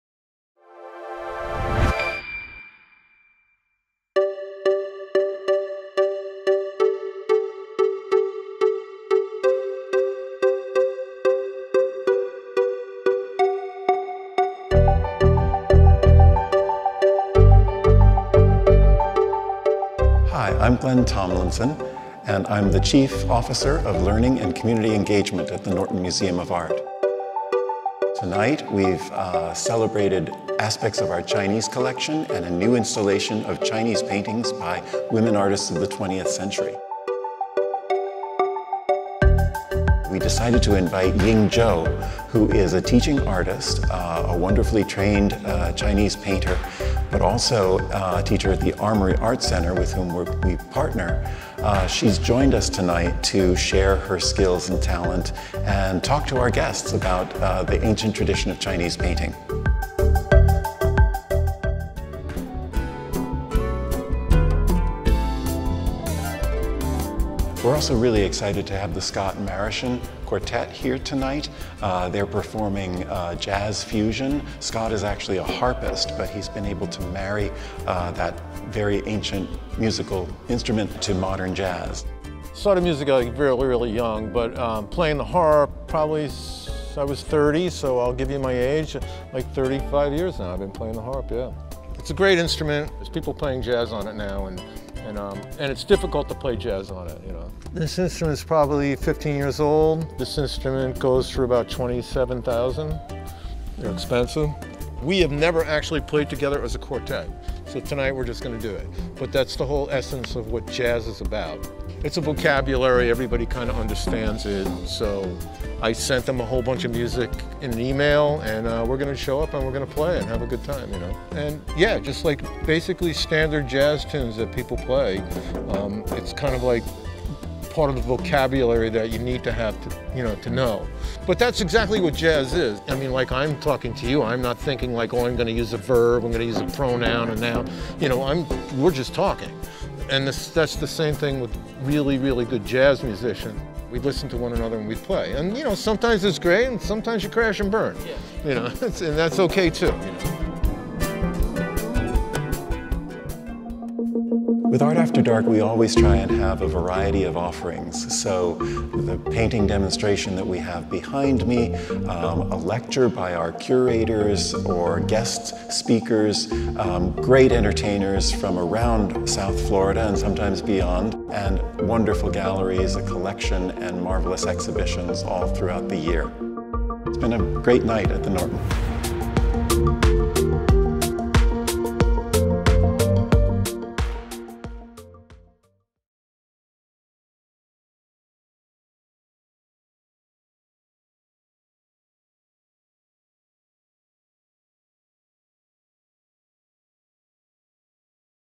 Art After Dark / 10.8.21
During Art After Dark on October 8th, Scott Marischen returned to the Norton to play his Electroacoustic Harp along with a group of very accomplished artists, including Tom Lippincott on guitar, Ettiene Fuentes on drums and Agustin Conti on acoustic bass. They performed Jazz, World Music, Popular & Latin songs. During our Open Studio workshop, guests were given the opportunity to learn the technique of Chinese brush painting with artist Ying Zhou from the Armory Art Center. Laurie Barnes, Elizabeth B. McGraw Curator of Chinese Art, offered a Curator Conversation about The Other Half of the Sky, the latest installation in the Niblack Gallery. Enjoy!